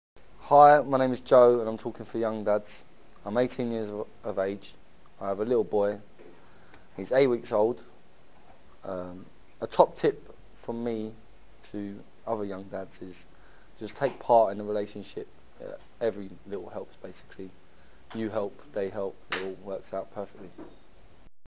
A relationship needs you to take part.
This is Joe, a young dad from Woolwich (London). He thinks that you need to really take part for your relationships to work.
Joe is supported by Working With Men & was filmed by other young dads in a session  powered by YoungDads.TV.